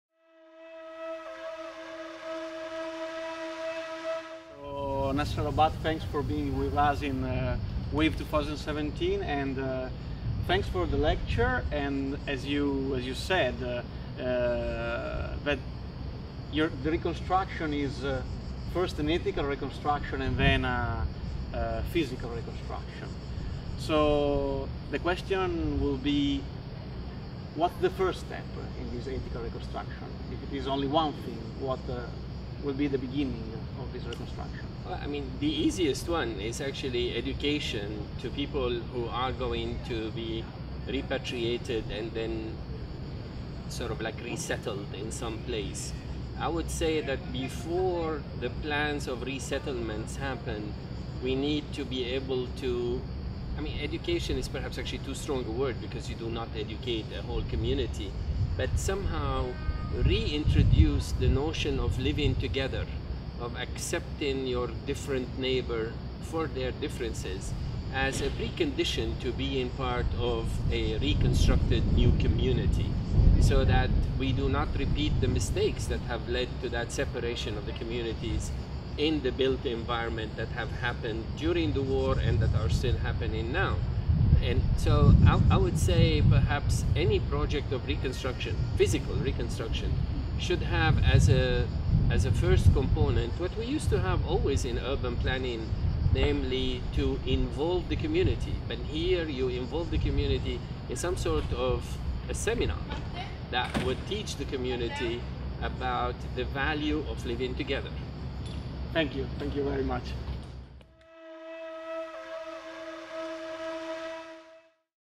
W.A.Ve. 2017 - Università Iuav di Venezia - Nasser Rabbat
W.A.Ve. 2017 | Syria - The making of the future

Interview with Nasser Rabbat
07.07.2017